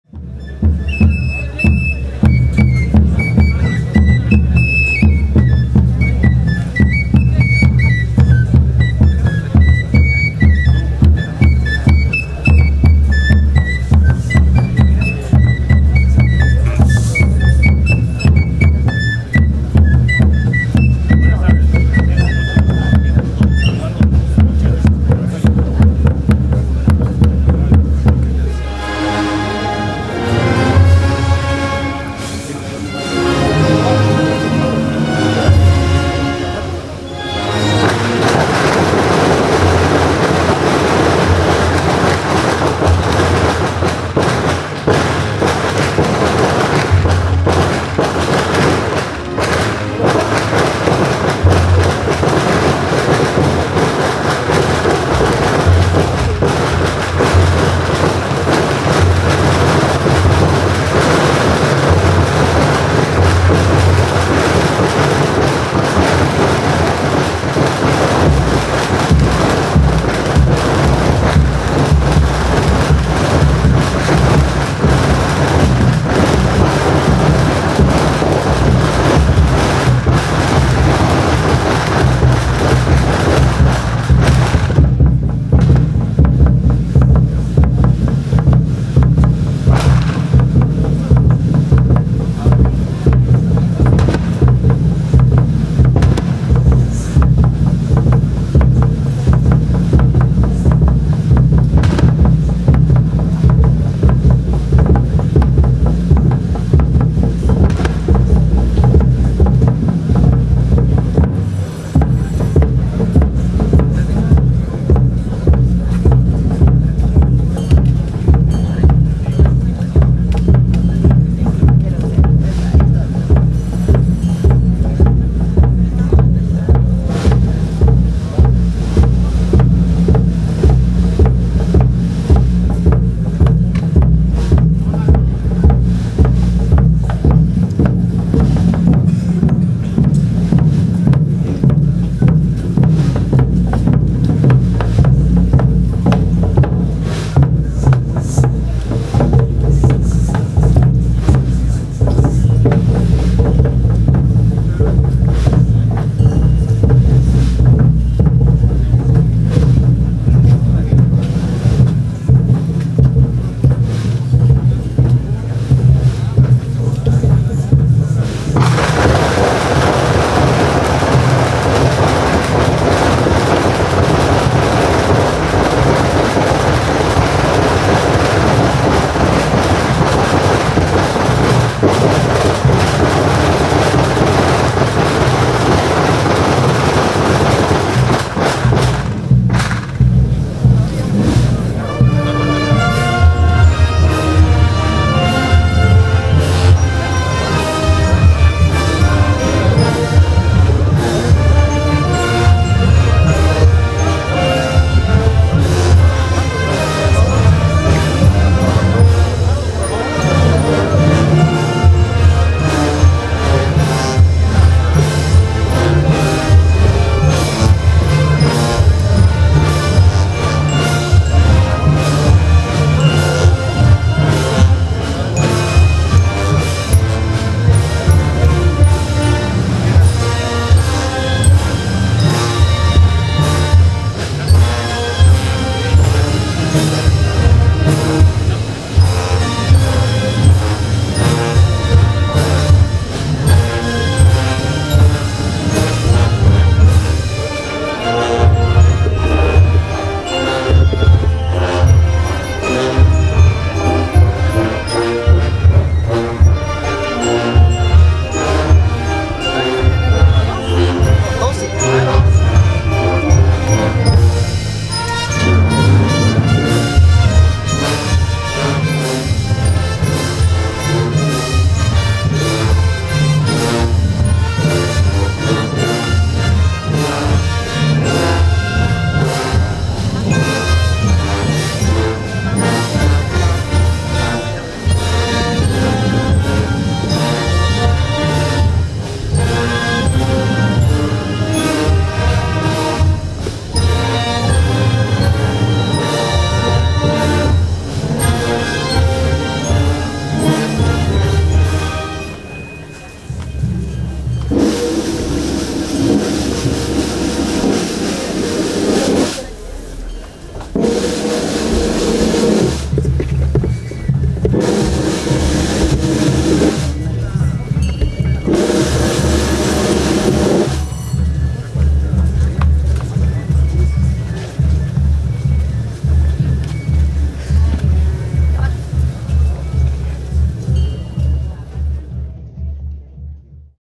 Salida Historica procesión de Peregrinación Bodas de Plata Consagrción del Patriarca San Jose 2024
Festiva procesión de la consagrada imagen de El Patriarca Señor San José en su histórica vista a la ciudad colonial de la Antigua Guatemala por motivo de sus Bodas de Plata de Consagración.